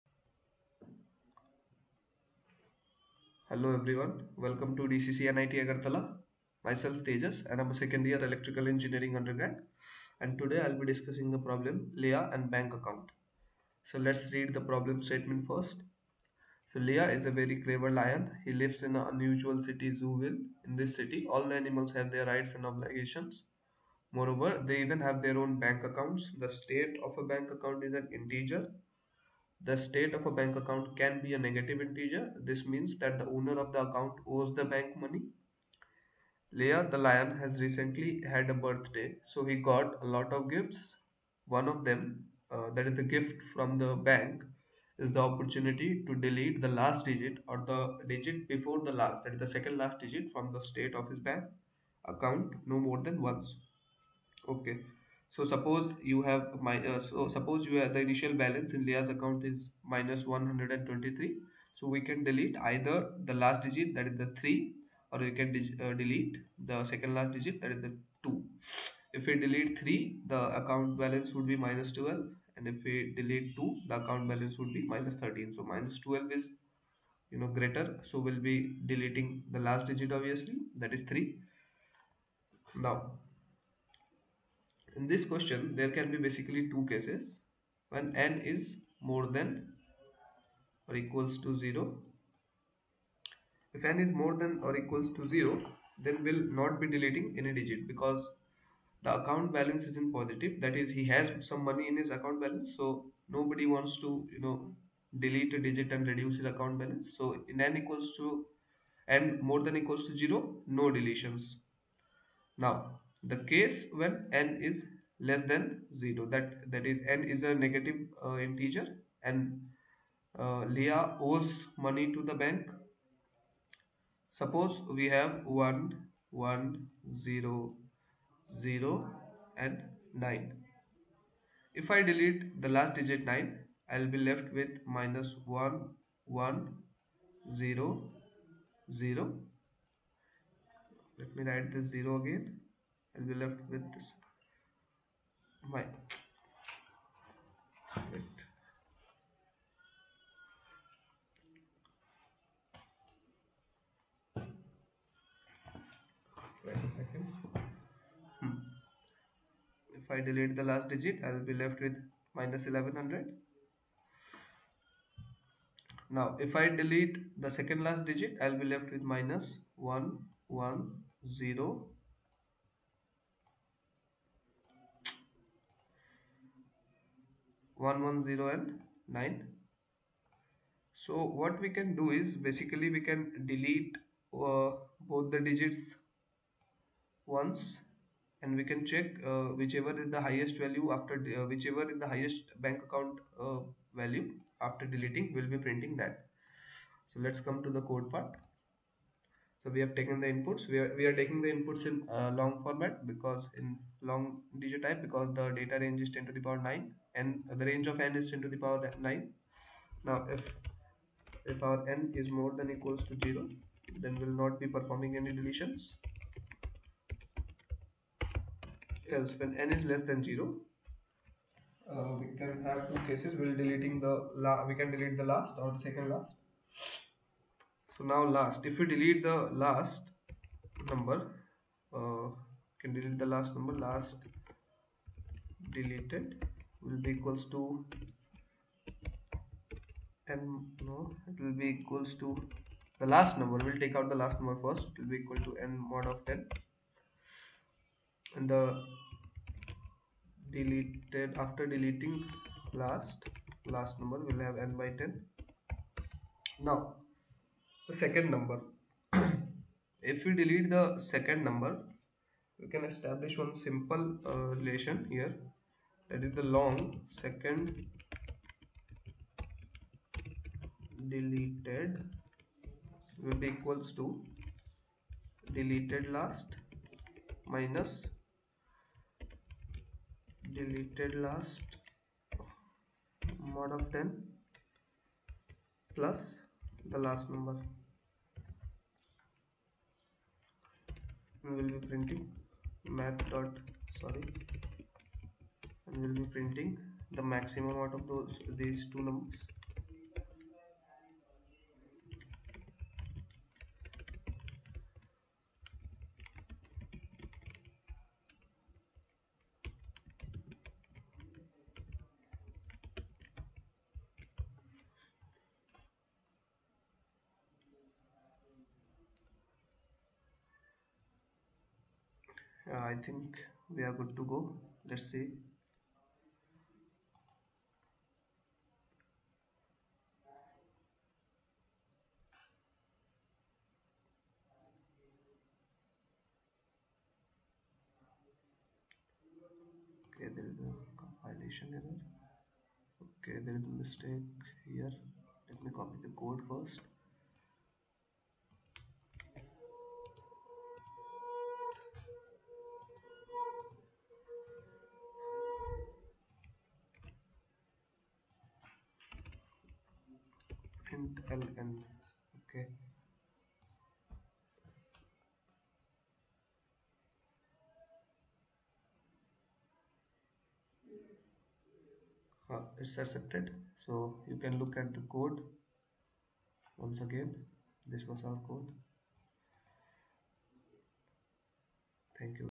A. Ilya and Bank Account | Codeforces Round 186 (Div. 2) | DCC NIT-A
Codeforces Round #186 (Div. 2) || A. Ilya and Bank Account || Tejashwadeep Jha(EE'25)

Happy Coding!